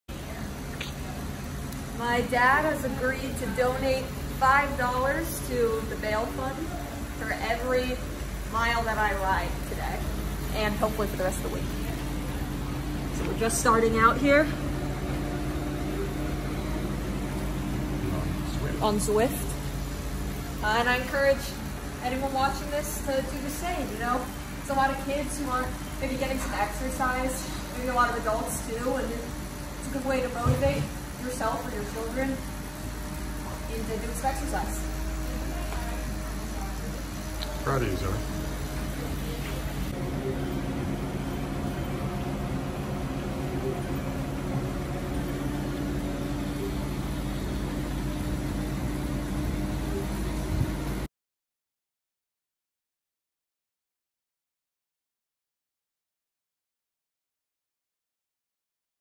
Zoey Raises Money for Black Lives Matter
This video is about riding zwift to raise money for BLM and the Protest Bail Funds.  This video has inspired Ric and Katy,  Leslie and Gary, Rob and Abby and Stephanie to also donate!  Zoey says tomorrow she will donate to a different BLM organization .